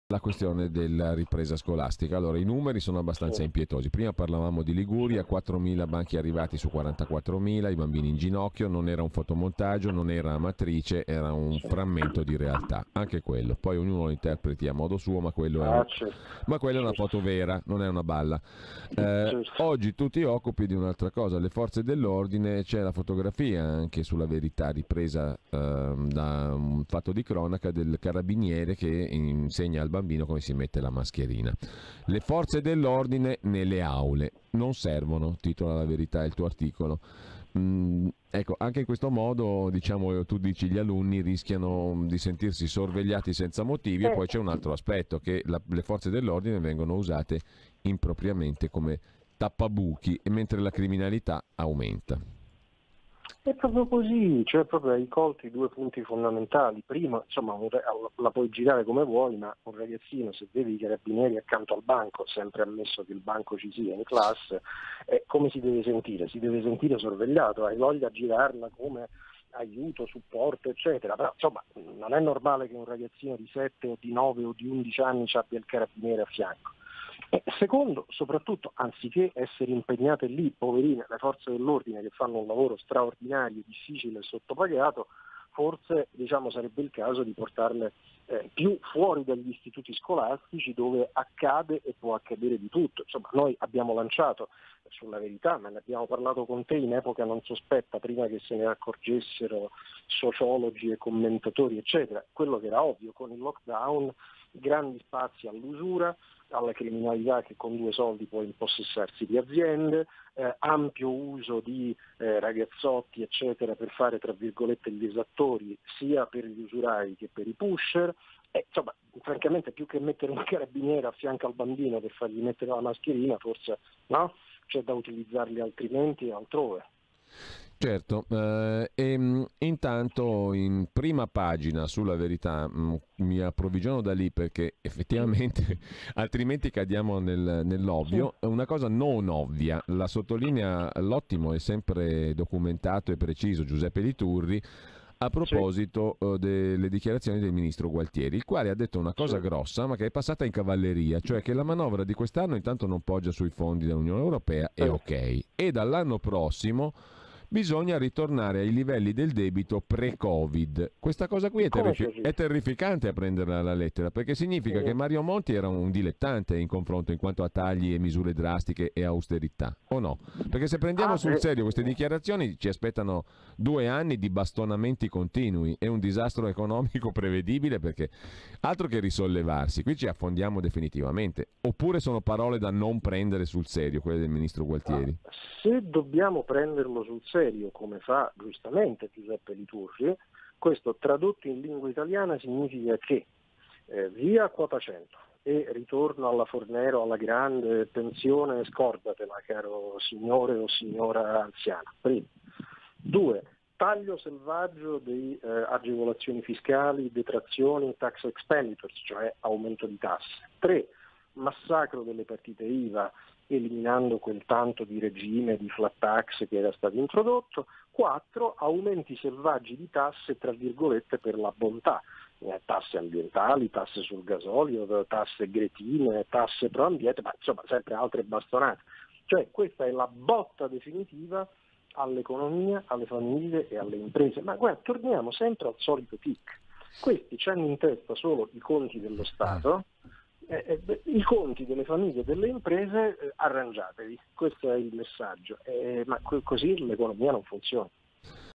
Capezzone.Covid19:Carabinieri a scuola,mentre Gualtieri promette austerity per rientrare del debito
Capezzone insieme a GiulioCainarca spiega la trovata assurda di mettere i Carabinieri a fianco dei bambini per le misure anti Covid19.
Intanto il ministro dell'economia fa una dichiarazione che passa inosservata riguardo una probabile austerity per rientrare del debito pubblico